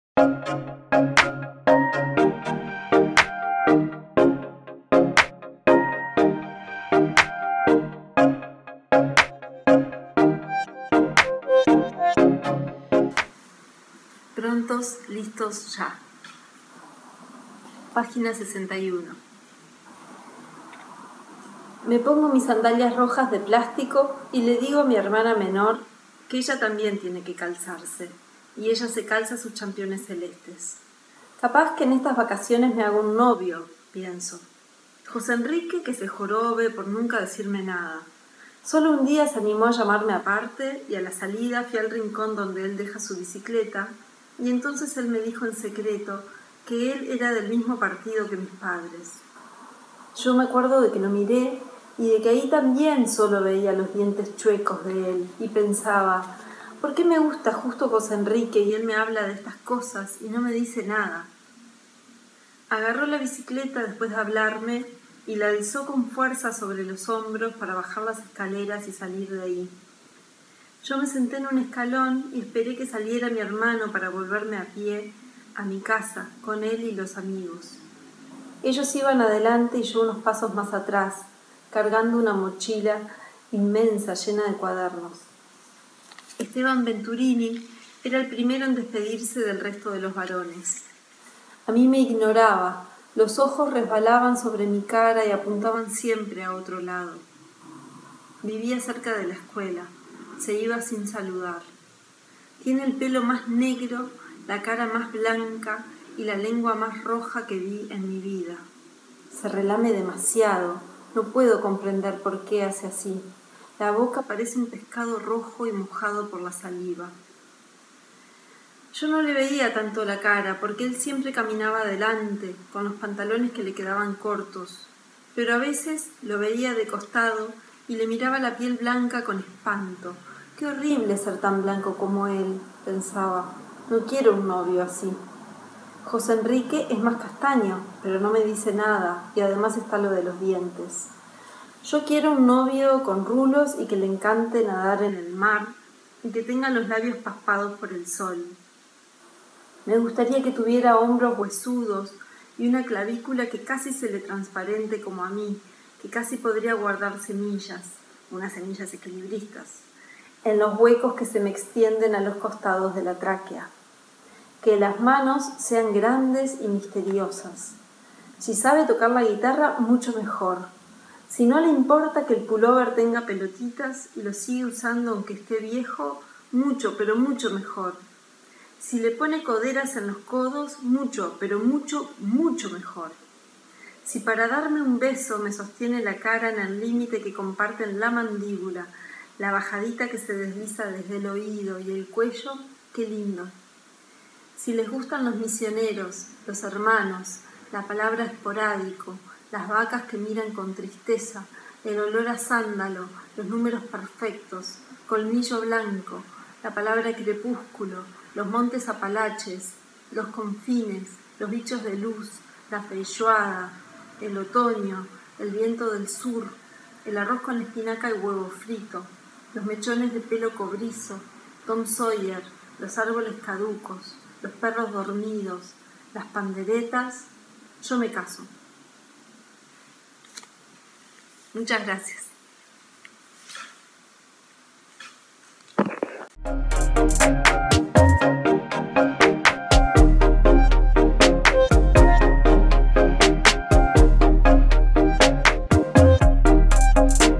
Inés Bortagaray - Prontos, listos, ya
Inés Bortagaray (Salto, 1975)

Guionista y escritora. Trabajó en guiones de largometrajes uruguayos. Por el guión de Mi amiga del parque en 2016 obtuvo el Premio Especial del Jurado en el Festival de Sundance. Varios de sus relatos aparecieron en antologías nacionales e internacionales.

Prontos, listos, ya (sinopsis): Un viaje a la playa narrado desde el asiento de atrás del auto familiar se vuelve un paréntesis en el espacio y el tiempo. Es un libro exquisito y luminoso que, a más de diez años de su publicación original, conserva la potencia de una voz única en la literatura contemporánea.

Intendencia de Montevideo. Abril 2020